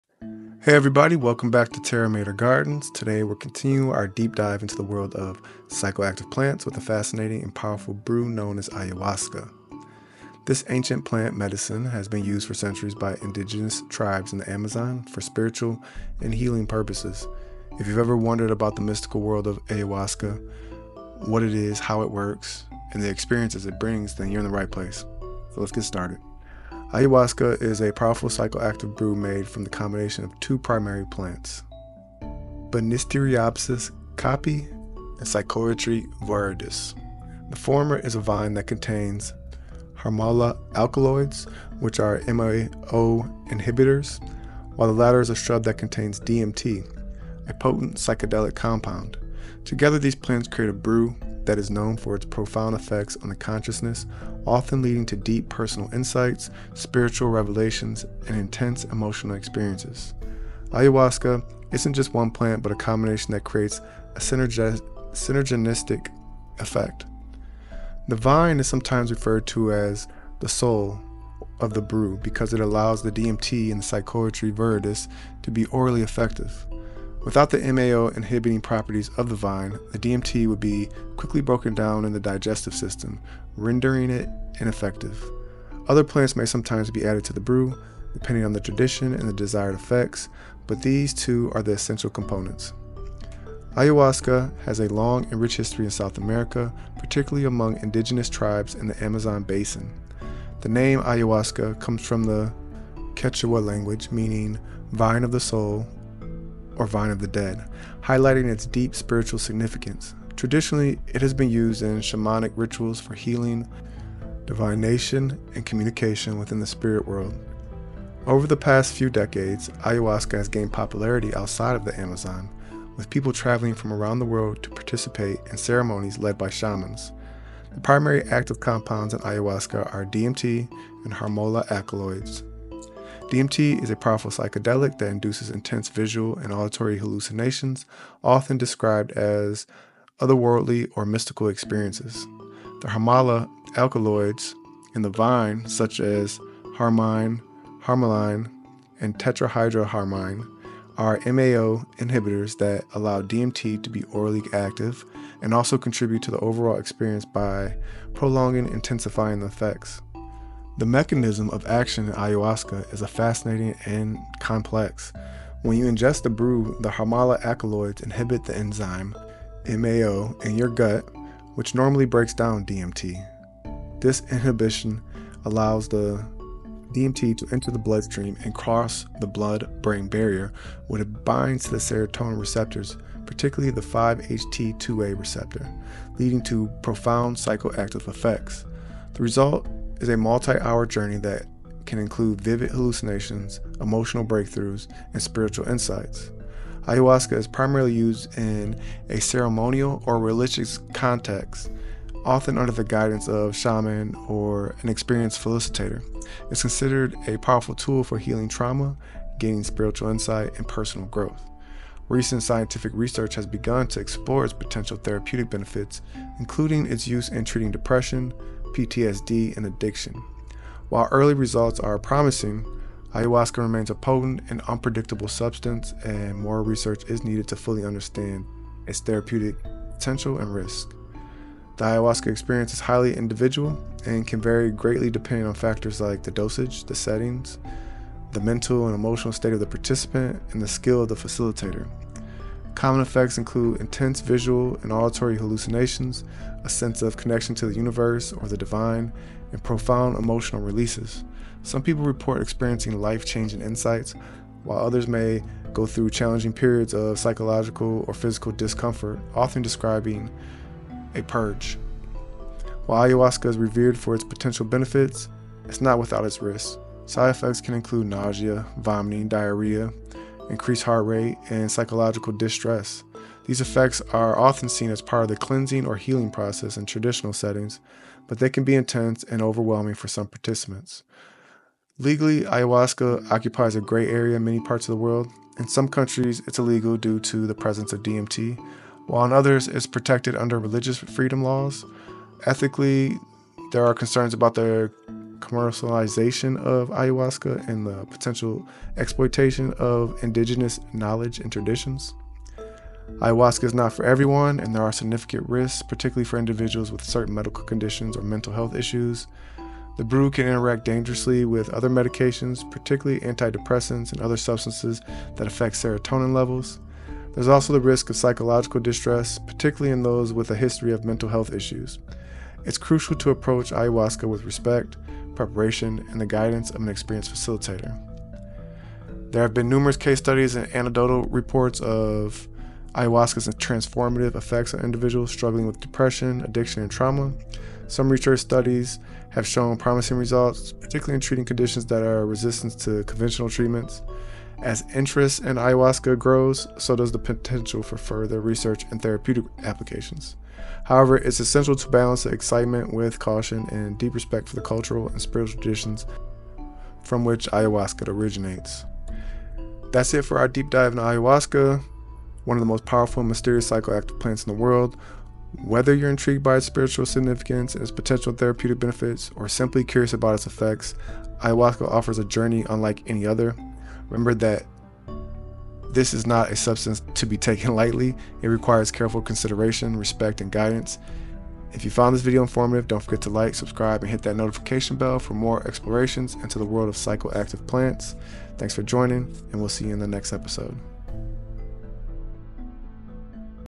🍃 Ayahuasca: Journey into the Spirit World with the Sacred Vine 🌿✨
Embark on a profound journey with Ayahuasca, the sacred psychoactive brew that has been used for centuries in Amazonian shamanic rituals. In this video, we delve into the mystical origins of Ayahuasca, composed of Banisteriopsis caapi and Psychotria viridis, and explore its powerful effects on consciousness, healing, and spiritual awakening. Learn about the traditional ceremonies, the role of shamans, and the science behind Ayahuasca’s transformative experiences.